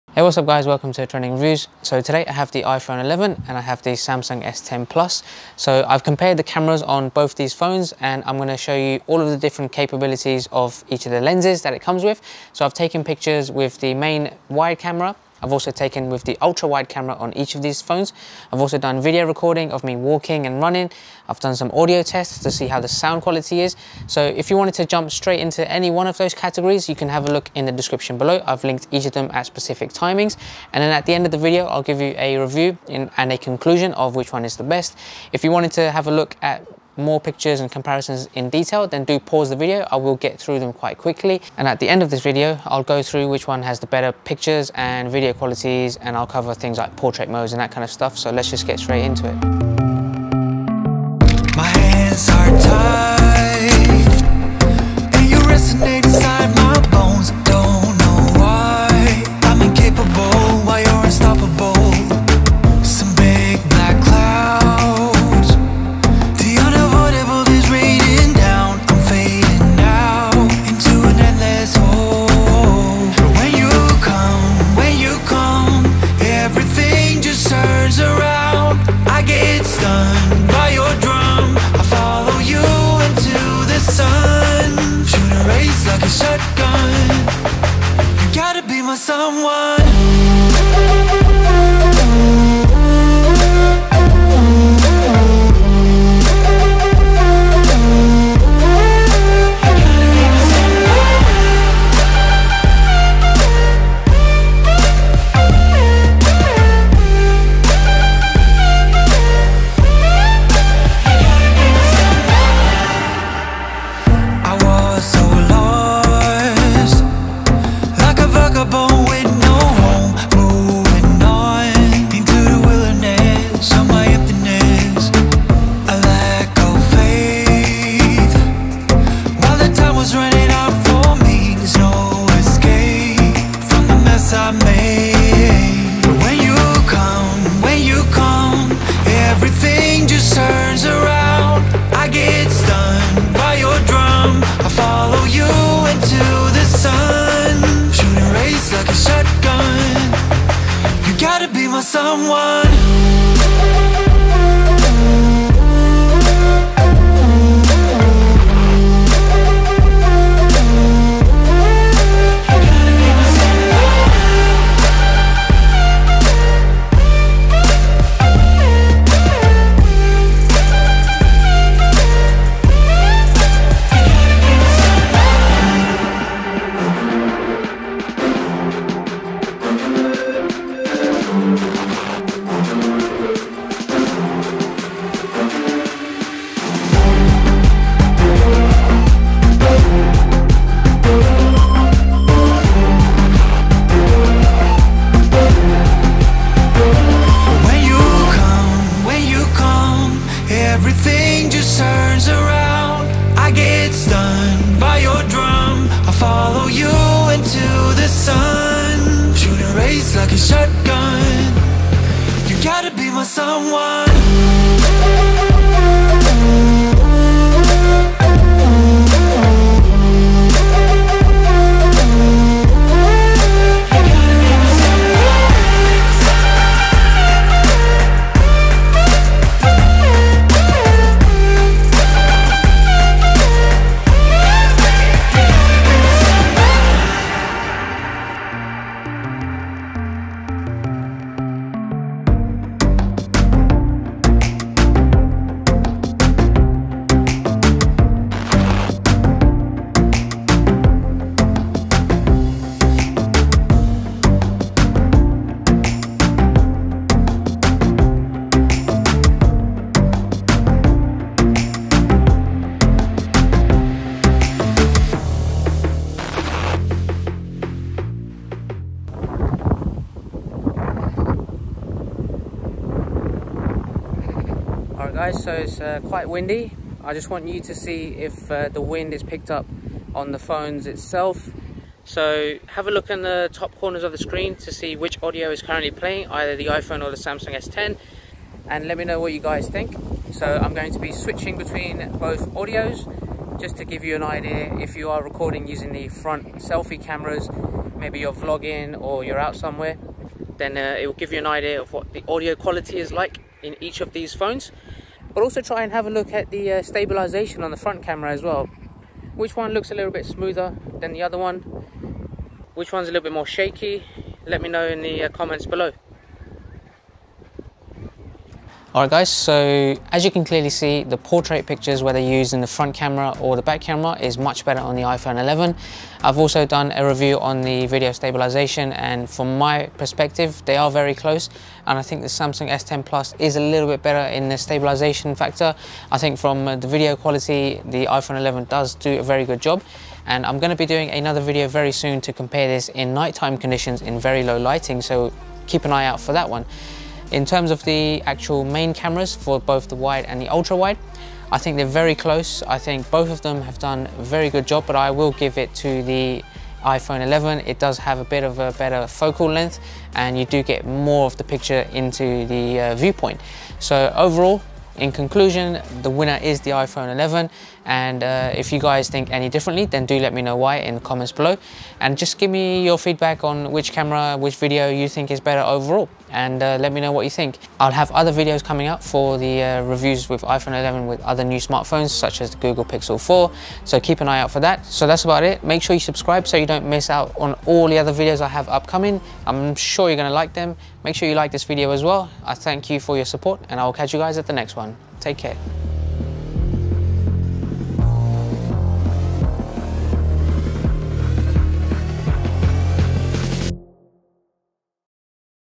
iPhone 11 vs Samsung S10+ (Photos, Videos & Audio Comparisons)!
Want to see the camera capabilities between the iPhone 11 and the Samsung S10+? Not sure which phone to buy? Then check out the differences in the following sections of my comparison:

00:59 - Wide vs Ultrawide Photos (Main Camera)
01:58 - Normal Wide Photos (auto)
02:18 - Wide vs Ultrawide Photos (Selfie Camera)
02:36 - Portrait Photos (Main back camera)
03:01 - Portrait Photos (Selfie camera)
03:12 - Video (Walking)
03:51 - Video (Running)
04:07 - Autofocus (Depth of Field) Macro Test
04:25 - Audio Tests